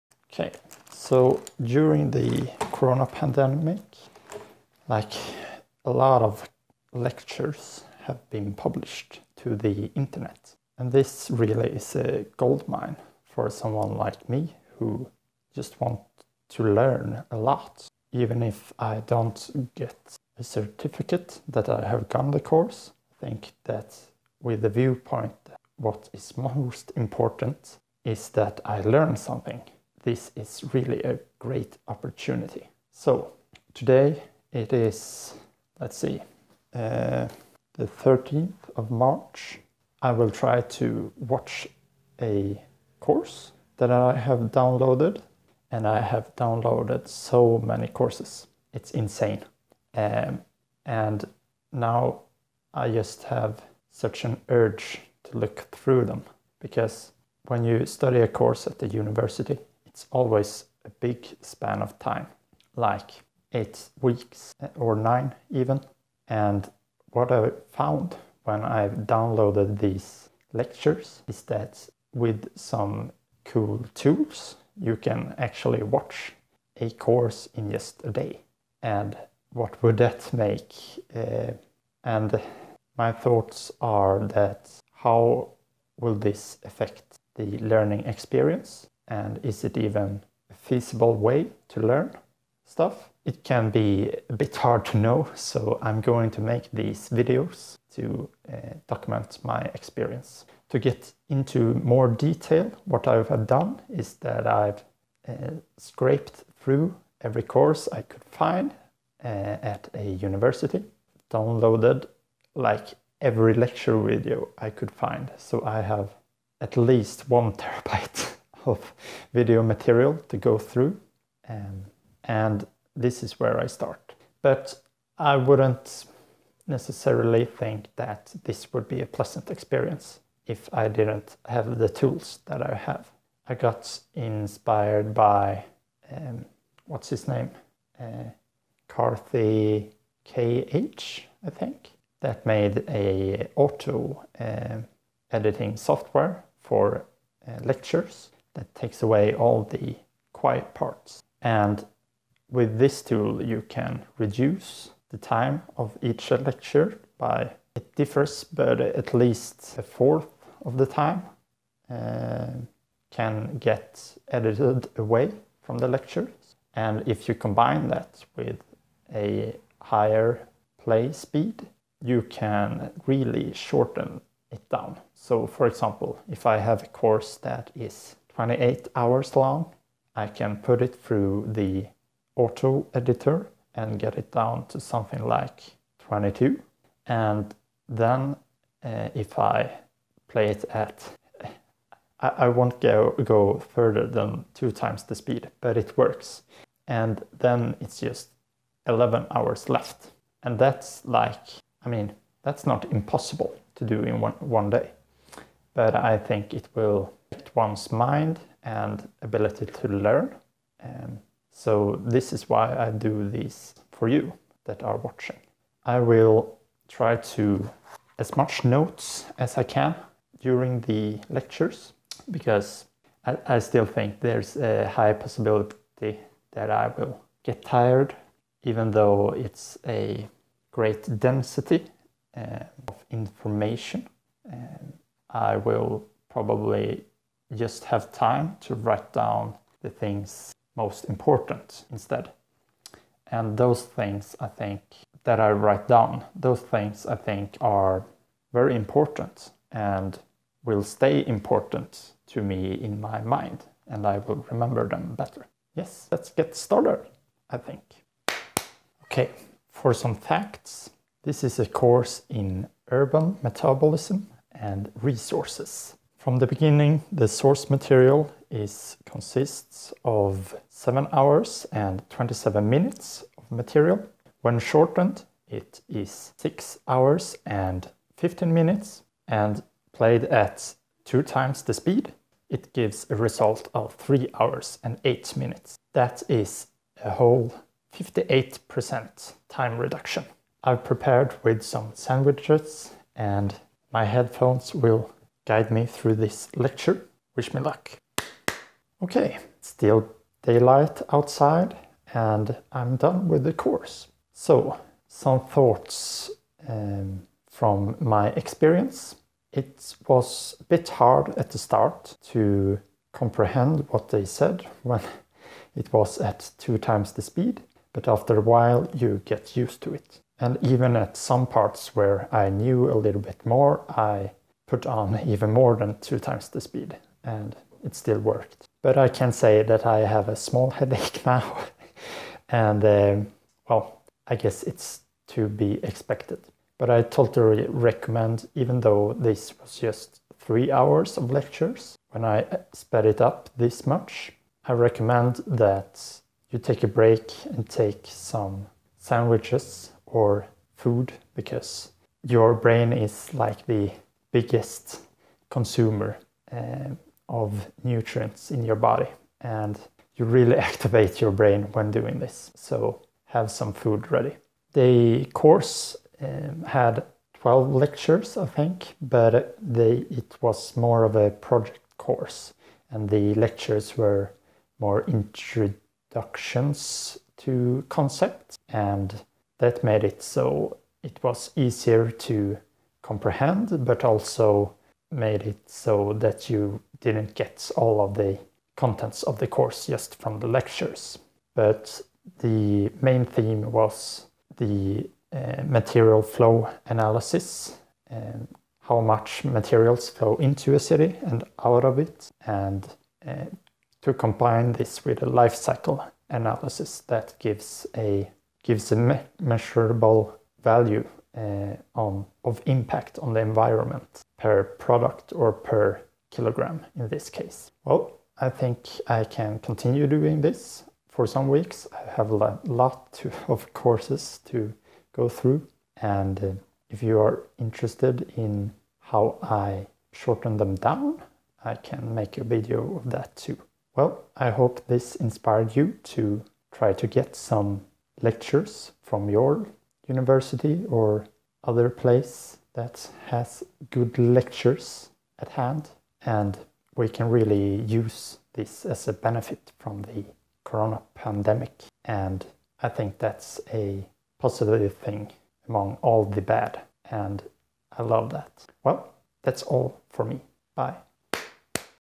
[001] Can You Watch all Lectures of a Course in a Day?
One of the benefits with the ongoing pandemic are all the lectures that have been published online. With some smart tools that can be used to watch a lot more of the material in a day than has ever been possible before. Is this the new way to study or just a worthless hassle? I try to find out.